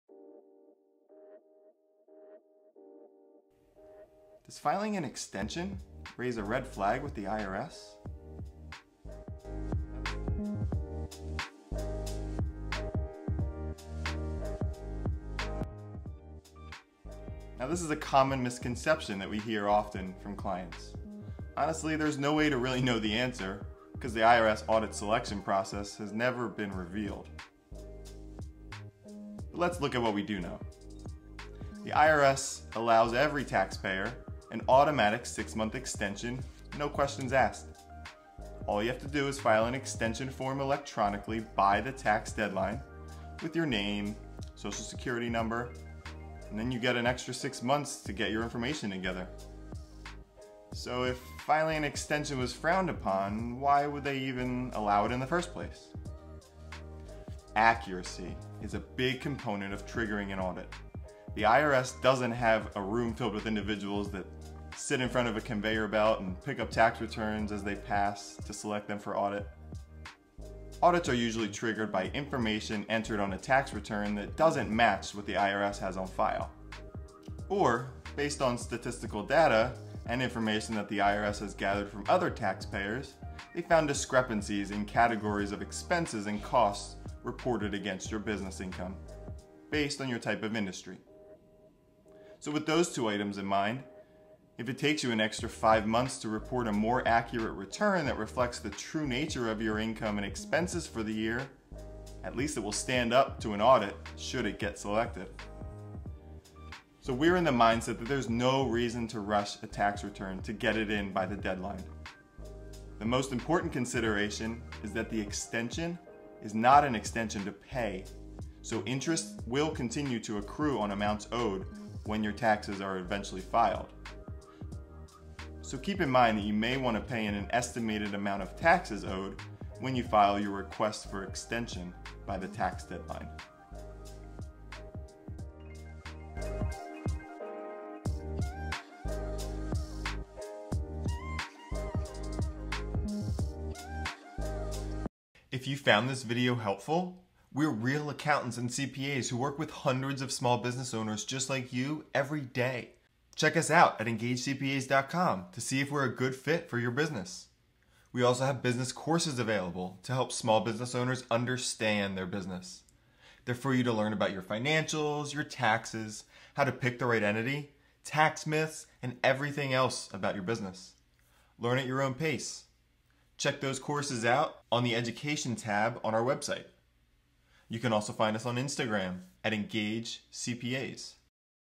Does Filing an Extension Raise a Red Flag with the IRS? - ENGAGE CPAs Education
This is such a common misconception about extensions. Extensions are granted so that individuals have more time to gather information so that they're able to file a more accurate tax return.

Business tax returns are also granted the same six month extension option, which also delays the preparation of those tax returns. So sometimes the extension isn't the fault of the individual because they have to wait for the business tax return to issue a K-1.

So extensions are no big deal and make no difference on whether or not you'll be targeted for an audit. But make sure you pay in any taxes owed by the April 15th deadline. Taxes are still due in April, even if you don't file until October. So if you owe in October, expect higher interest and penalties for paying late.